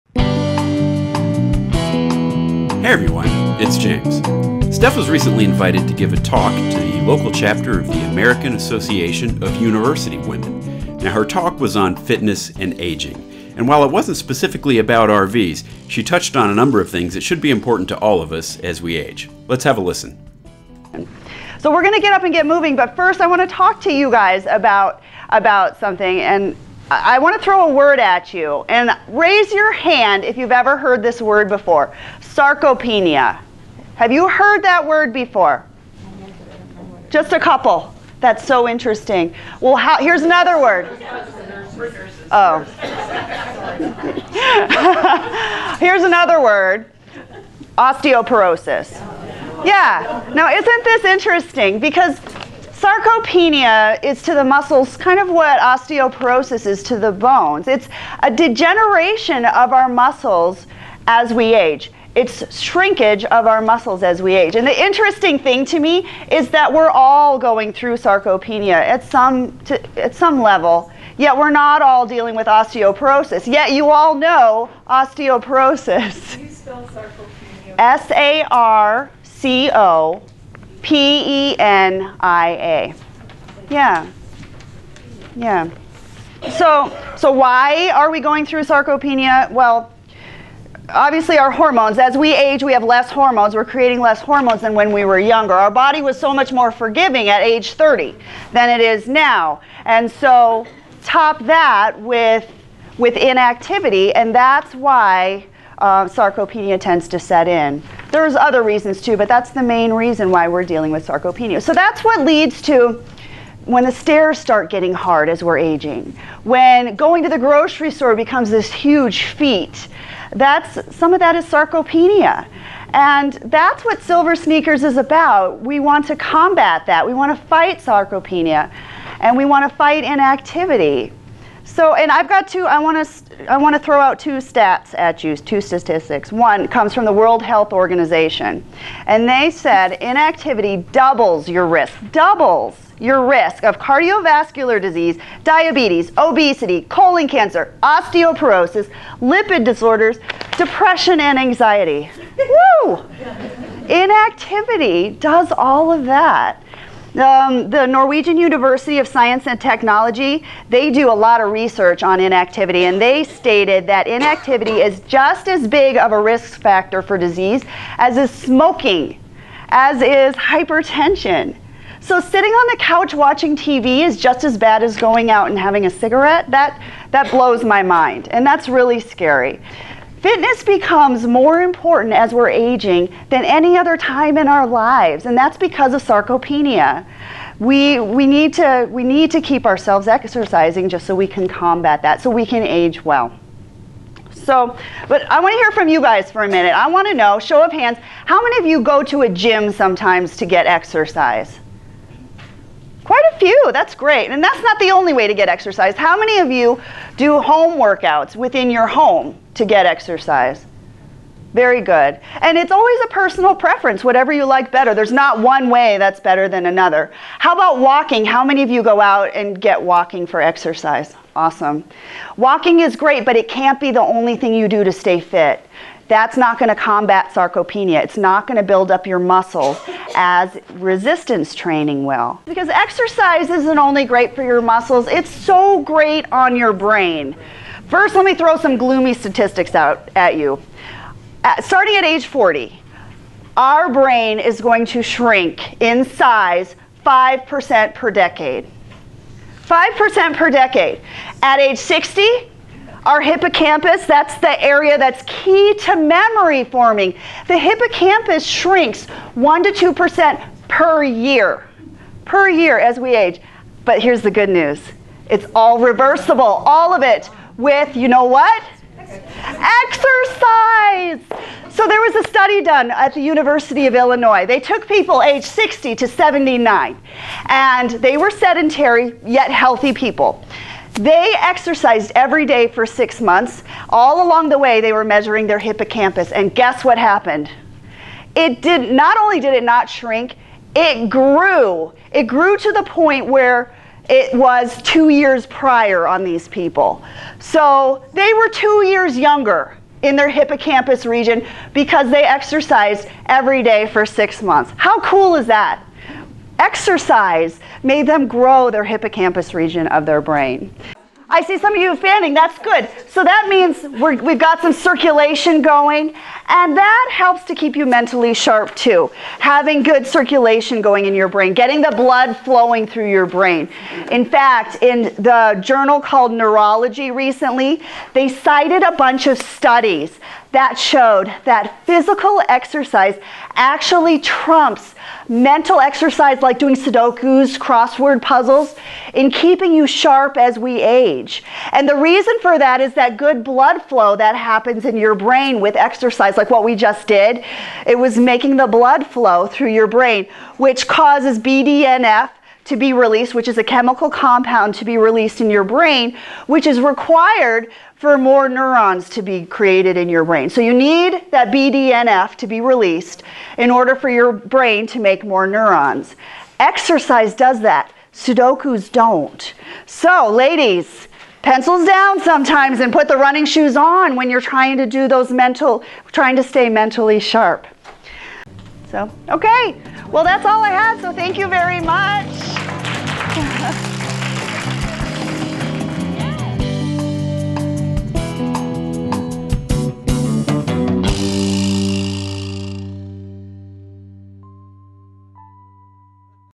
Healthy Aging - Stef talks Fitness with the AAUW
- There's more than just the videos!

Stefany was recently invited to give a talk on fitness and aging to the Salt Lake City chapter of the American Association of University Women.  Though the talk wasn't about RVing, Stef cites a number of studies, and tries to dispel a few myths about exercise as we age.  It really was pretty interesting.

Do watch the video -- it's only a few minutes, and it just might change how you approach your golden years.